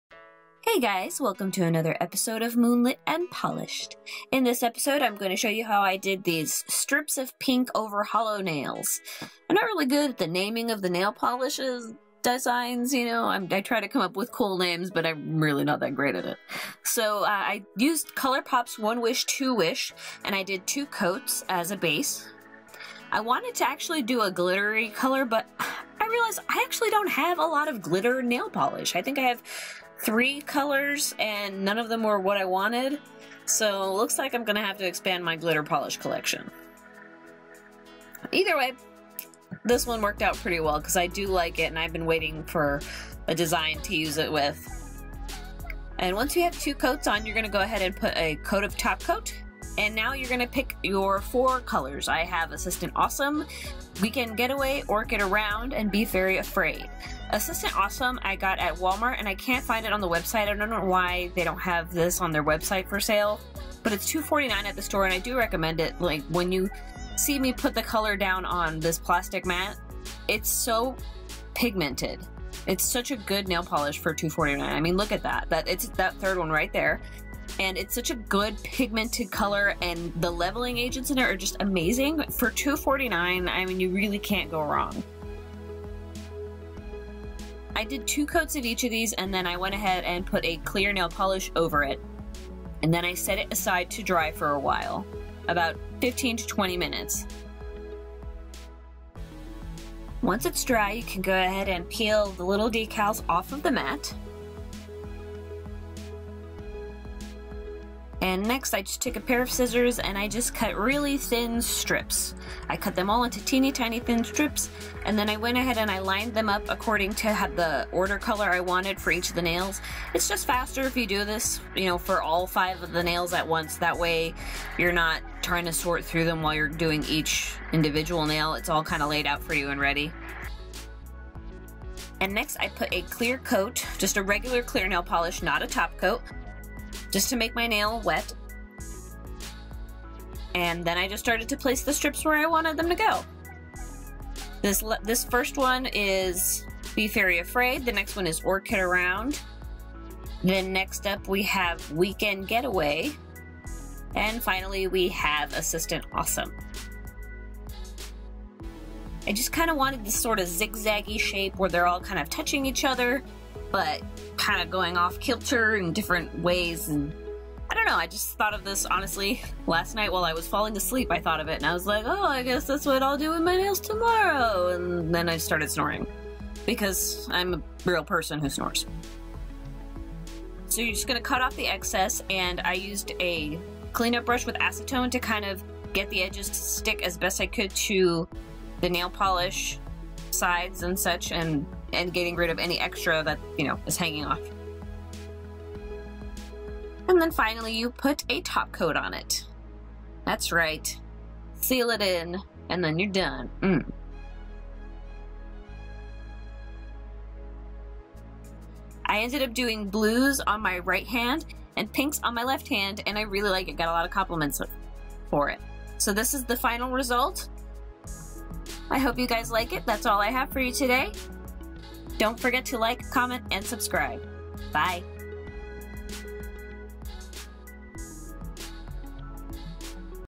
Strips of Pink Over Holo
Hey guys!
Here is another video where I show you all some nails. Yay! I finally able to find the P2 Volume glass on walmart’s site so I was able to link it for you guys 😊

Assistant Awesome $2.94